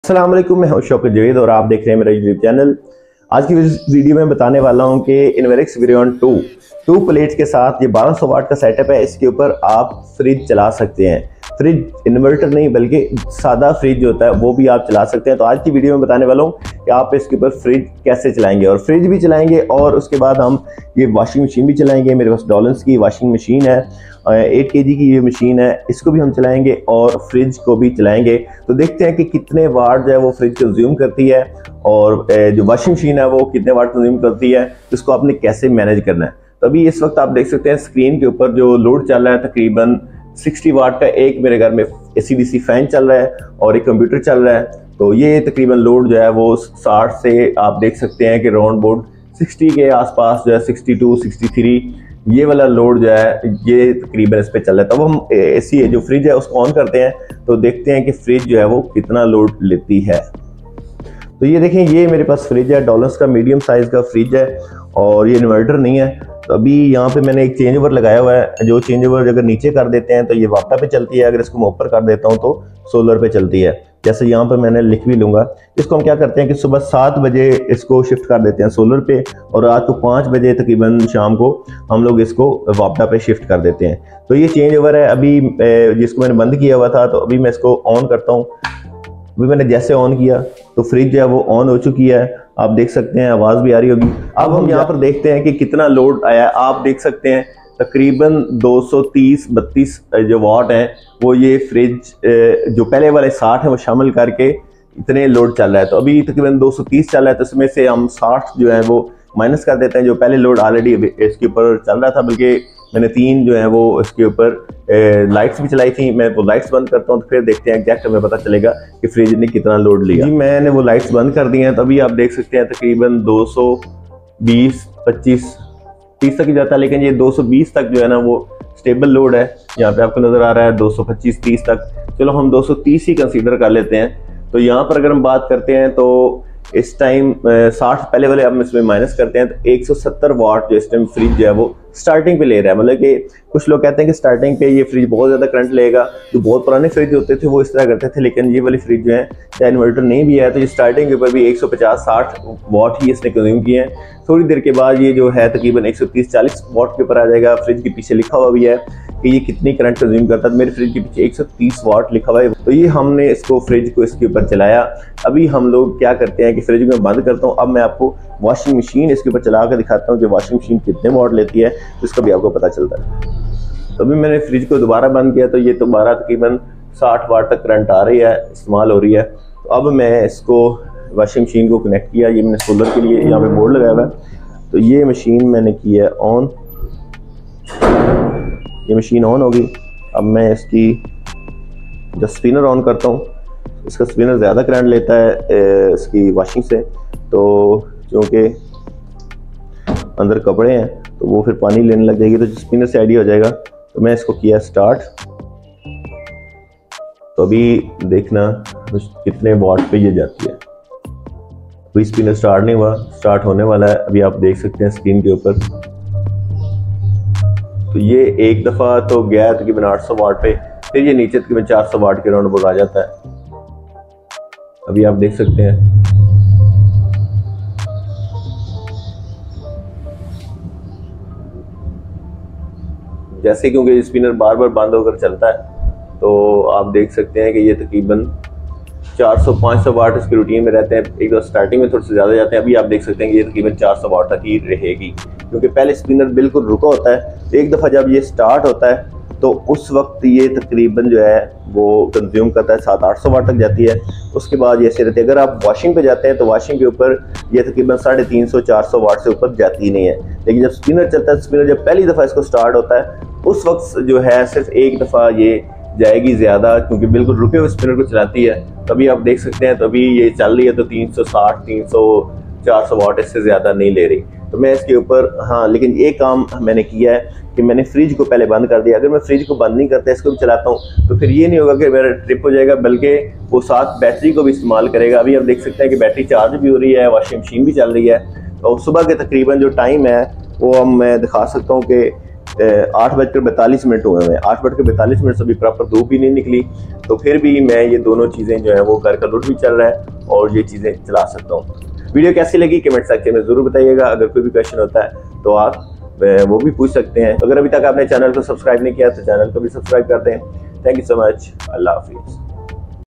Fridge and Washing Machine Live Load Test on Inverex Veyron 2 1200w || Shokat Javed
Hey there! Today, we're diving into an exciting live load test featuring the Inverex Veyron 2 1200w inverter. Join me as I put a fridge and washing machine to the test, running them on this powerful inverter.

We'll be exploring how well the Inverex Veyron 2 1200w handles the load of these household appliances in real-time. From power consumption to performance, we'll uncover everything you need to know about running your appliances on this inverter.

If you're in the market for an inverter or simply curious about its capabilities, this video is for you. Sit back, relax, and let's see how the Inverex Veyron 2 1200w handles the challenge of powering a fridge and washing machine simultaneously. Let's get started!